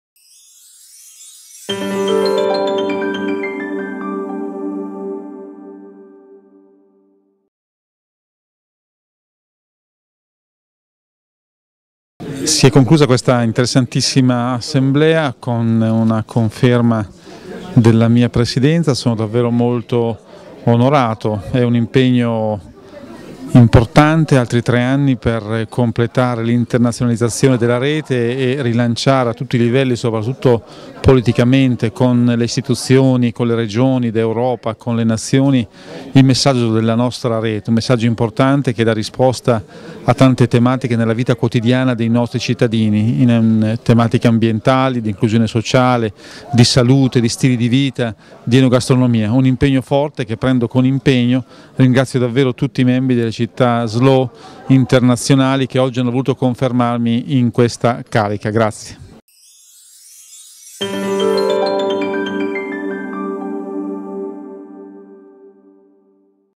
Cittaslow International Assembly 2011 - Presidente Gian Luca Marconi
Il Presidente Gian Luca Marconi riconfermato alla guida dell'Associazione Cittaslow International durante l'Assemblea Internazionale che si è tenuta in Polonia a Lidzbark Warminski il 24 e 25 giugno 2011